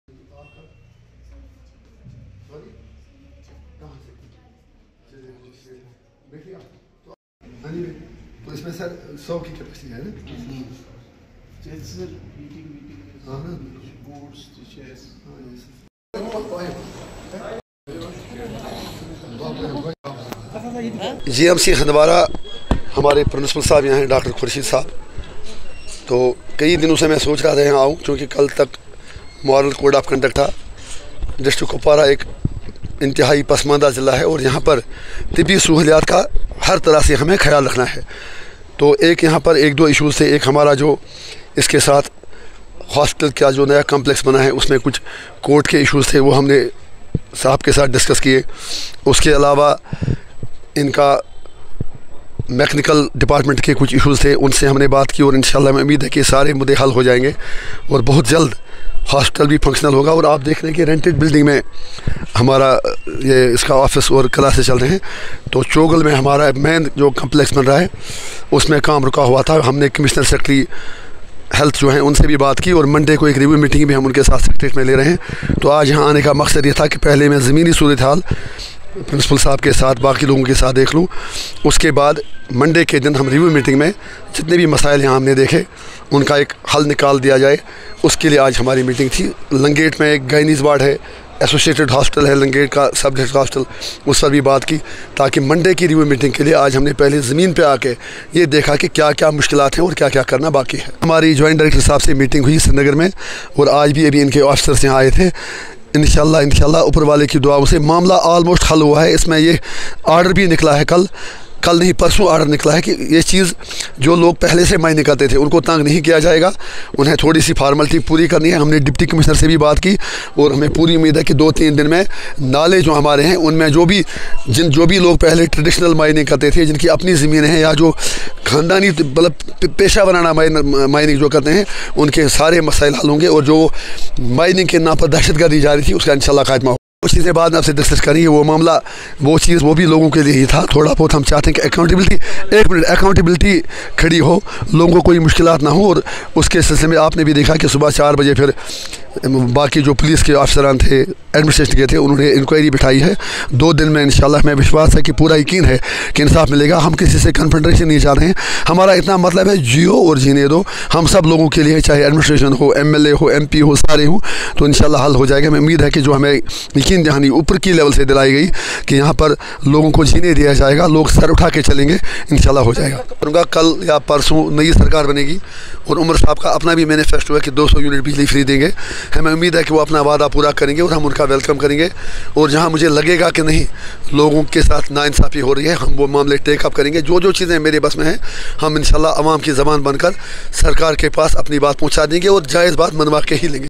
Engineer Rashid visted transit accomodation of Government Medical College Handwara
Engineer Rashid visted transit accomodation of Government Medical College Handwara and took stock of the situation. He held a meeting with the official GMC Handwara including principal Dr Khursheed and other senior officials.